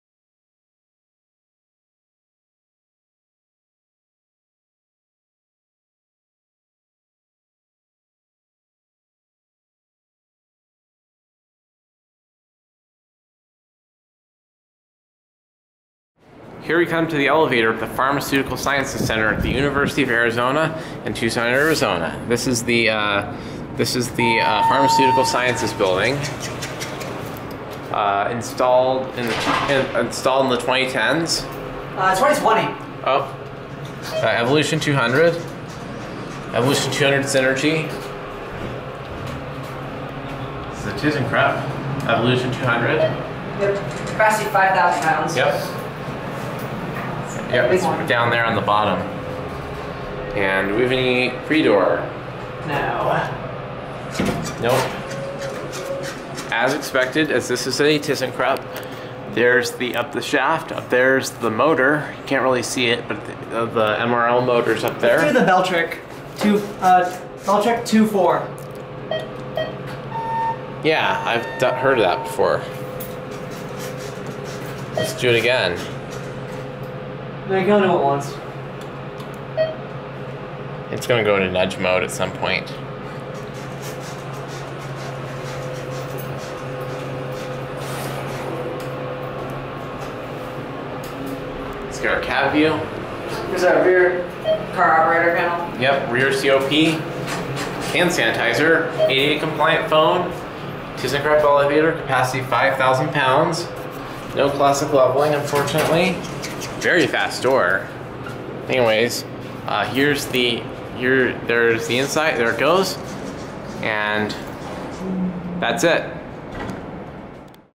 Awesome Thyssenkrupp Evolution 200 MRL Traction Elevator, Pharma. Sciences Bldg. U of A, Tucson, AZ
(Filmed January 12th 2024)

This is an awesome Thyssenkrupp Evolution 200 Elevator! This elevator runs well, is very fast, and has a rear door and COP. Other than that this elevator is nothing special, but it is very nice.

Recorded on my Sony DSC-HX99

Brand: Thyssenkrupp Elevator
Type: AC MRL Traction
Fixtures: MAD BS Thyssenkrupp Signa4
Controller Type: Microprocessor
Capacity: 5000 lbs.
Year Installed: 2020